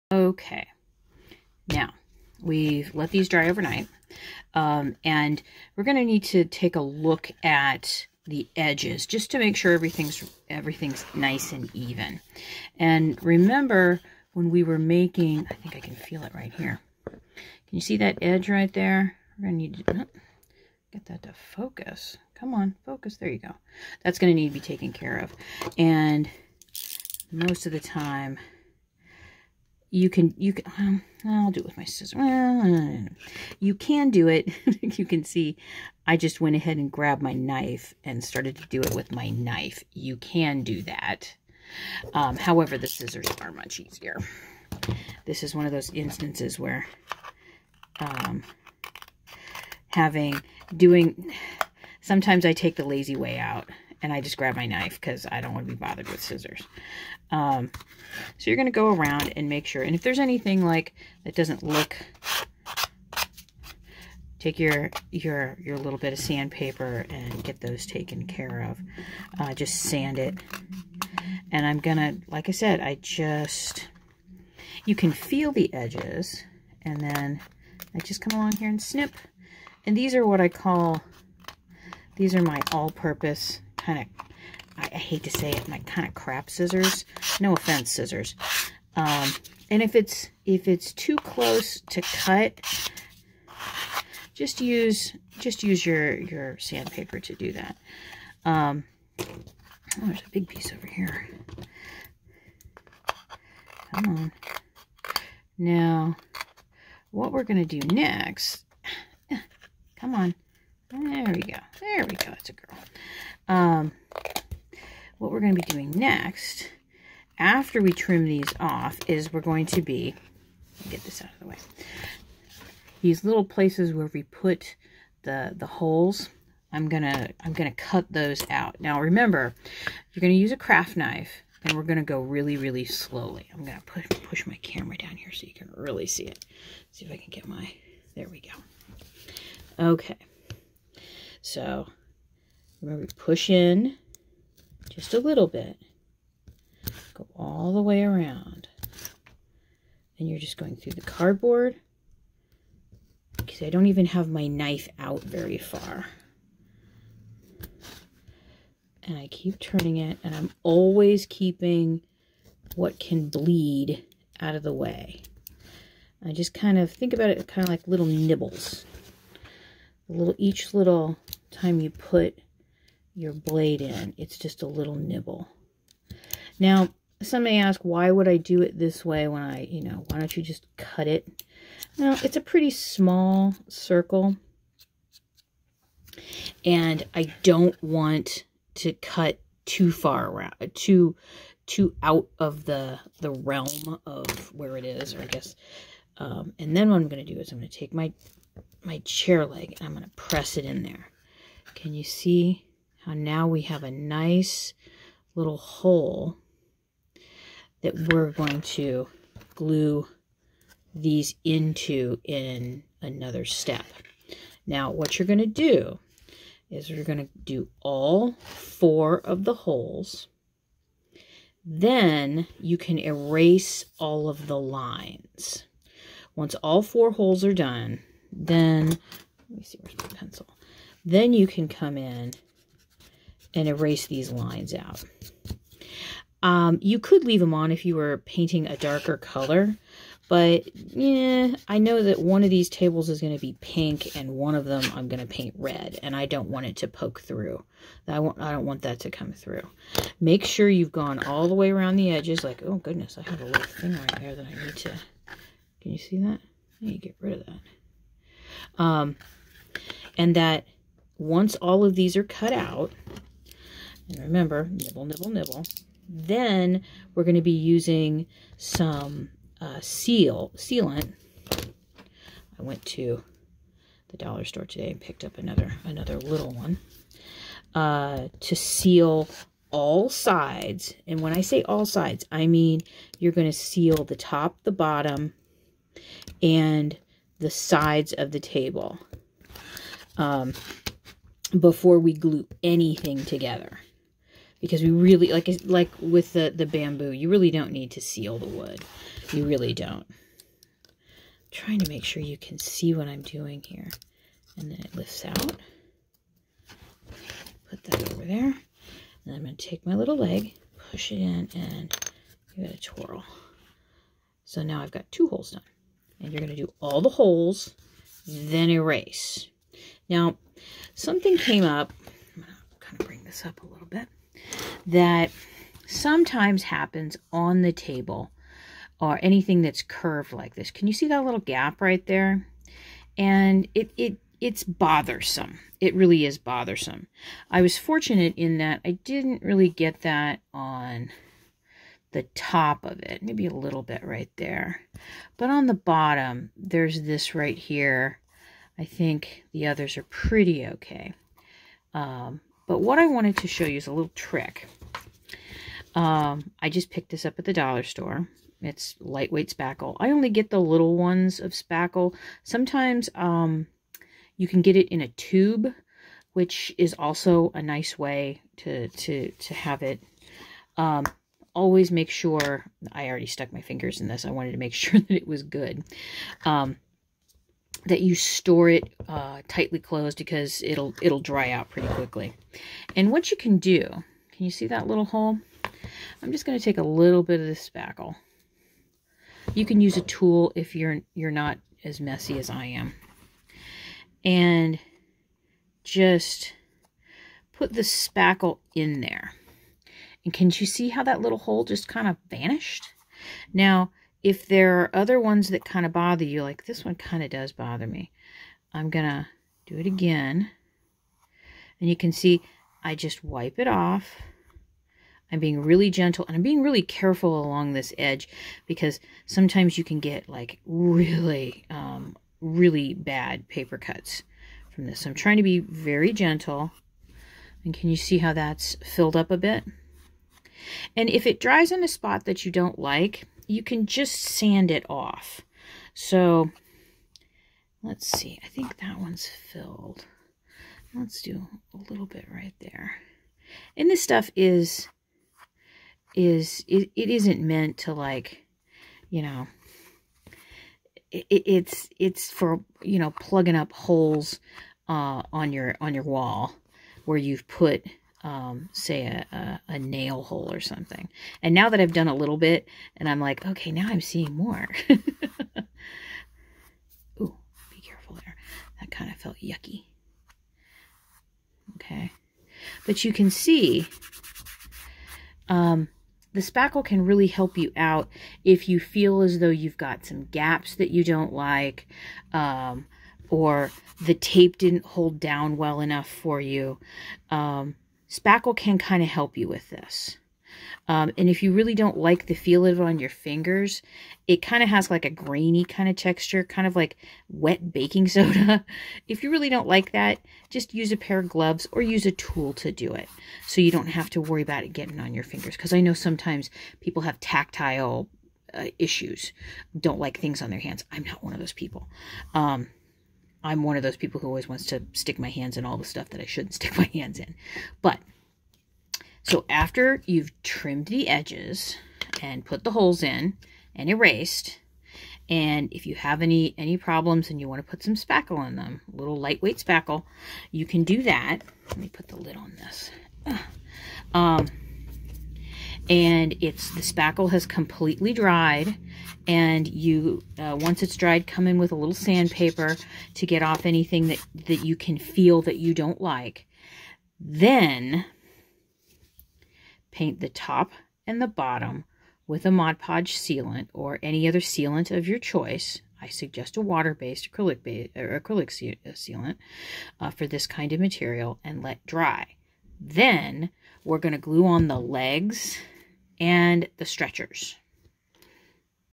Kitchen Table: Cutting Holes for Legs and Using Spackle (Video 24)
Again, cutting the holes for the table goes so much quicker than the chairs. After all, with the table, you only have to add four legs once.

Here's the link to an Amazon Wishlist I created so that you can see what the tools and materials look like. As well as if you would like to purchase the items: